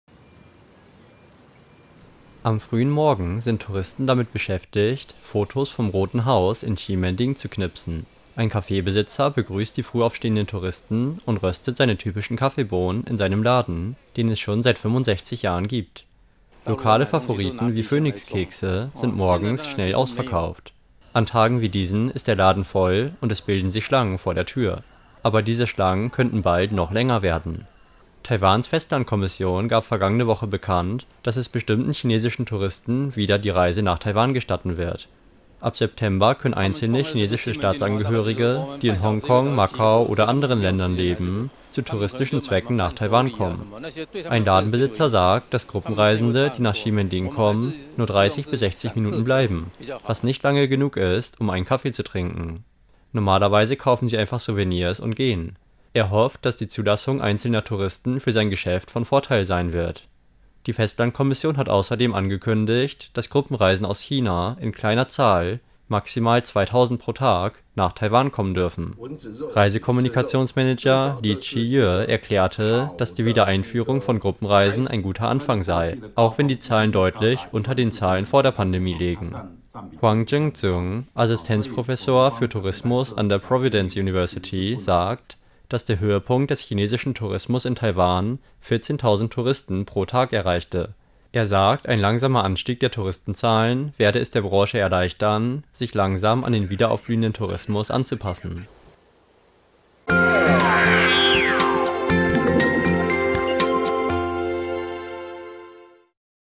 Festlandkommission kündigt Rückkehr von chinesischen Touristen an
Touristen vom chinesischen Festland dürfen Taiwan bald wieder besuchen. Experten und Geschäftsinhaber überlegen nun, wie sie vom Zustrom an Touristen profitieren und sich darauf einstellen können.